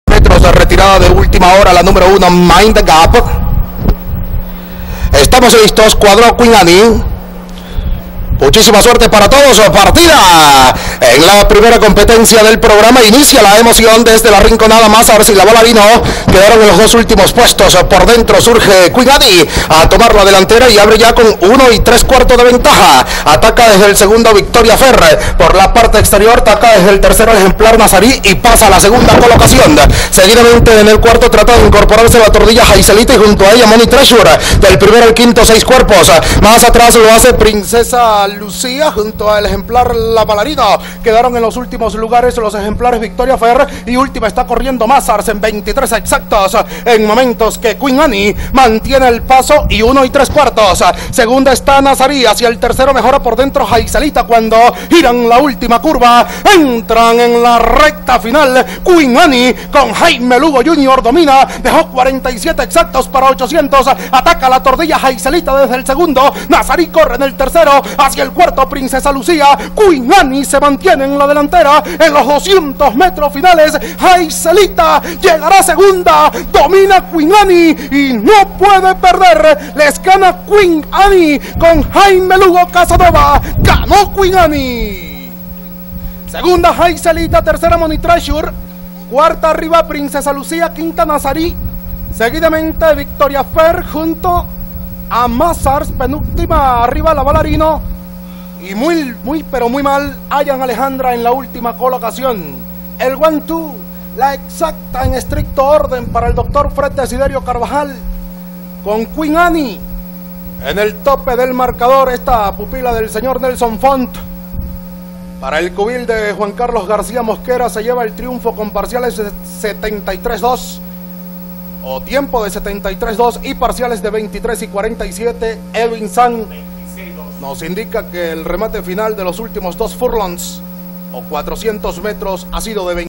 1era. Carrera | Queen Annie | Hipódromo La Rinconada | Reunión 13 | 2022
Con facilidad se llevó la victoria en la primera carrera la yegua 'Queen Annie' (2) por medio del arreo de Jaime Lugo Jr. y la preparación del líder de la estadística, Juan C. García.

➡️En el 2do. lugar arribó 'Jaicelita' (6).